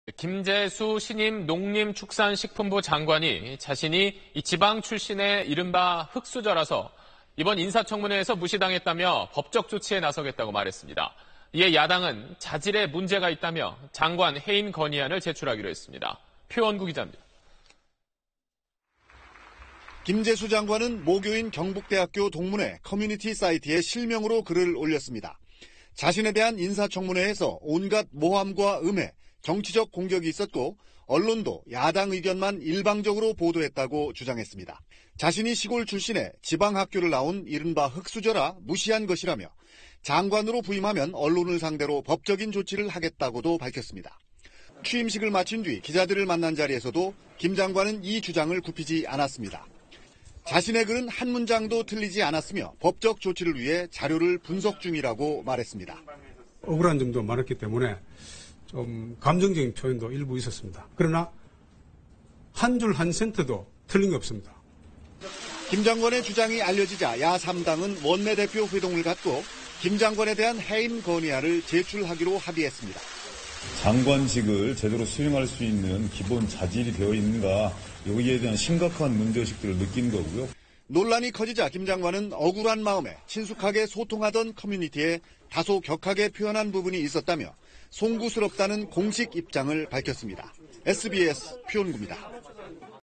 김재수 "흙수저라 무시"…야당, 해임 건의안 제출 / SBS
김재수 신임 농림축산식품부 장관이 자신이 지방 출신의 이른바 흙수저라서 이번 인사청문회에서 무시당했다며 법적 조치에 나서곘다고 말했습니다. 이에 야당은 자질에 문제가 있다며 장관 해임 건의안을 제출하기로 했습니다.